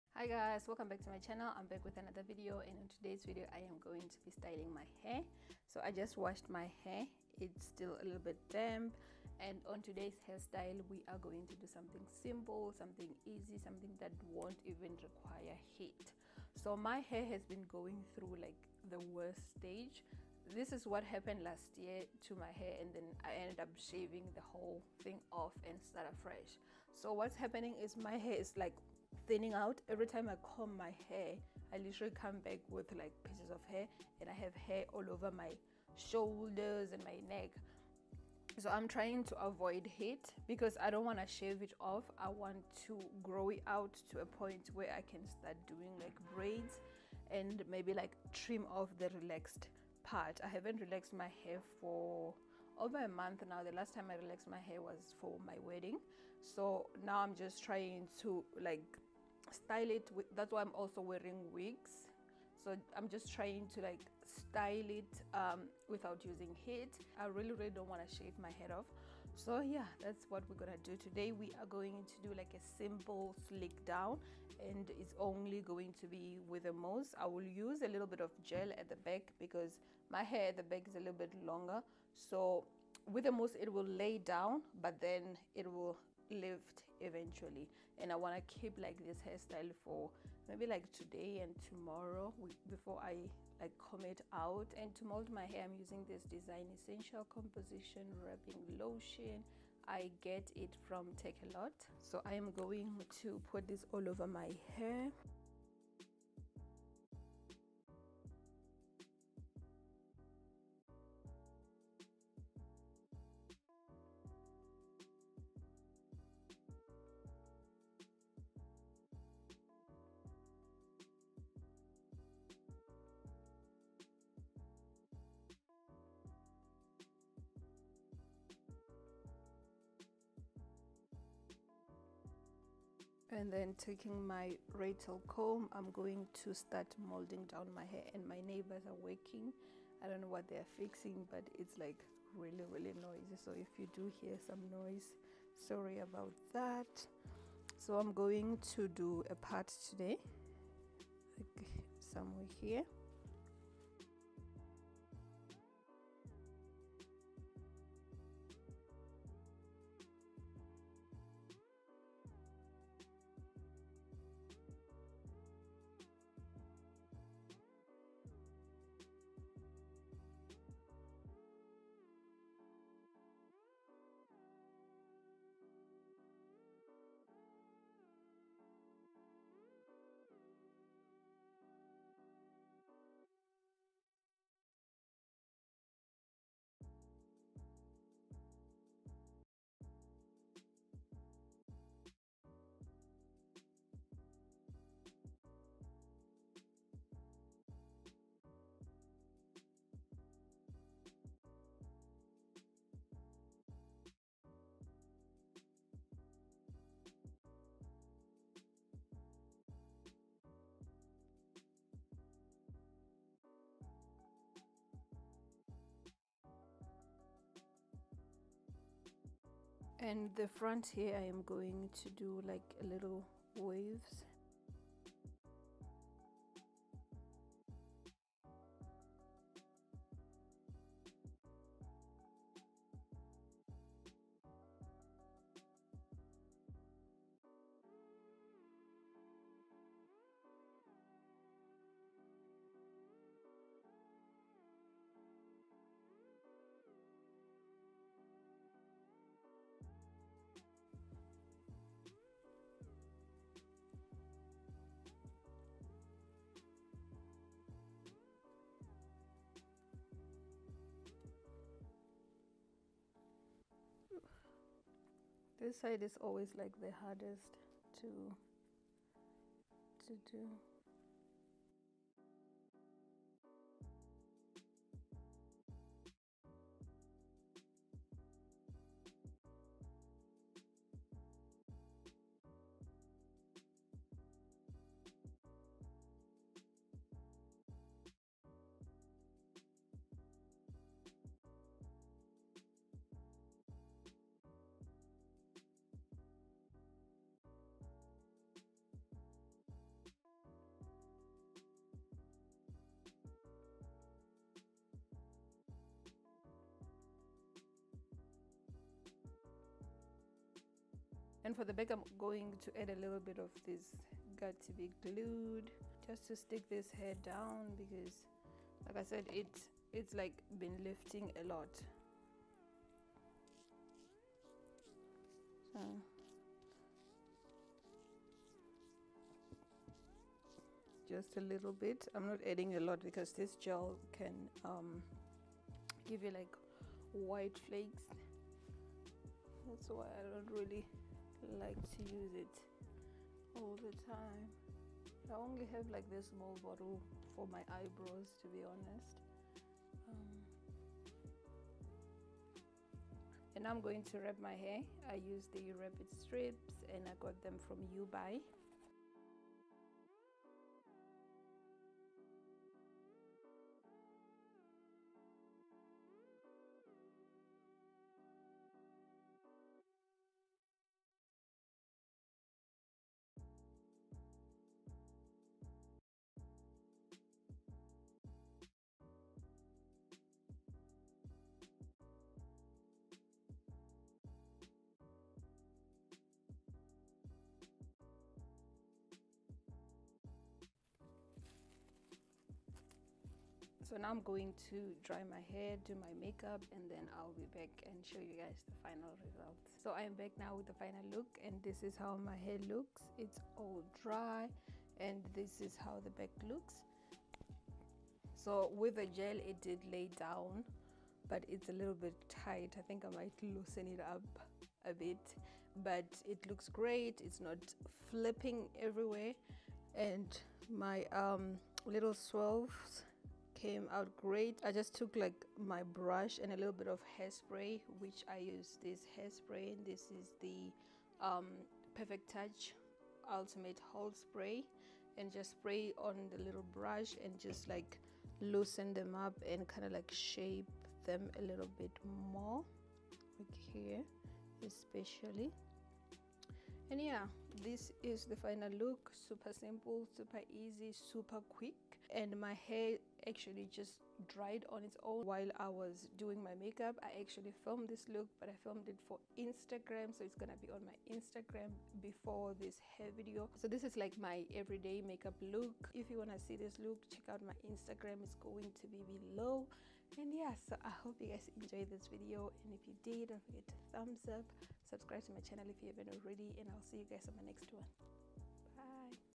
How to Sleek Pixie Cut Mold NO HEAT Styling| South African YouTuber
I N S T A G R A M: tondi_johnston
Hey, I thought I would come and show you my thought process when trying to get a sleek mold

PRODUCTS USED:
Design EssentialsCompositions Foam Wrap Lotion
Wrapp-it Styling Strips
Perfect Touch
Spray n Stay Hairspray Ultimate Hold

FILMING TOOLS:
Ringlight - Neewer